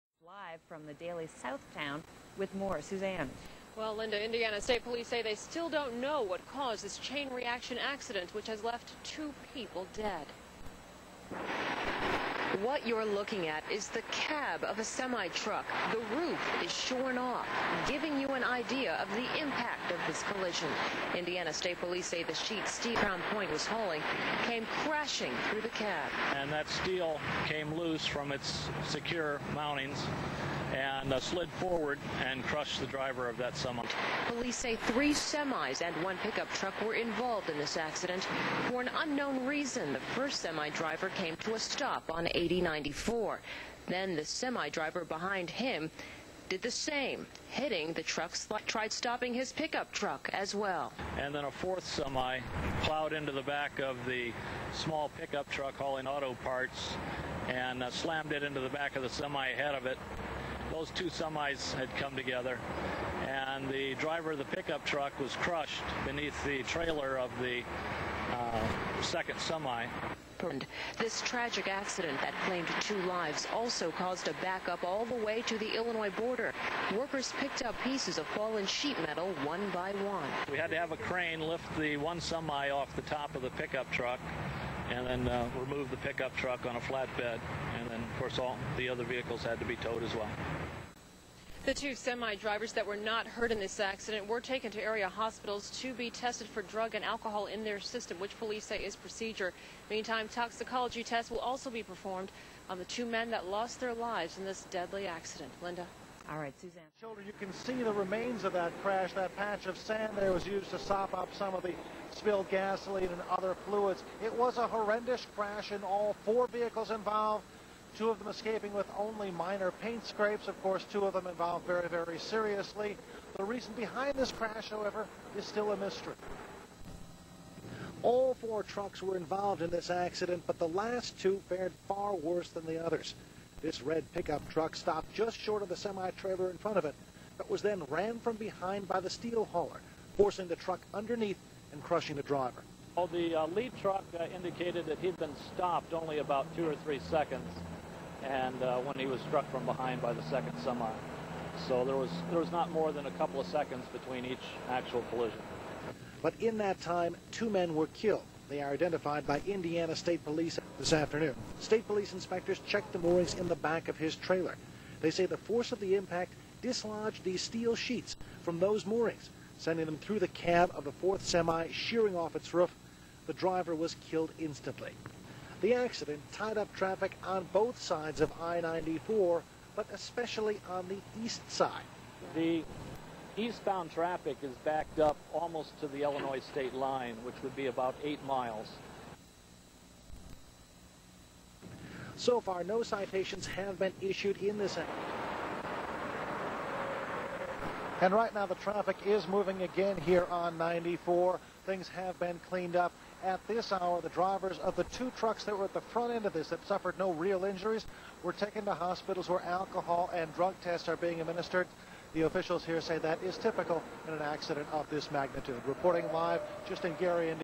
Waffco Heavy Duty Towing and Recovery
Accident Interstate 94 at Burr st. Gary, In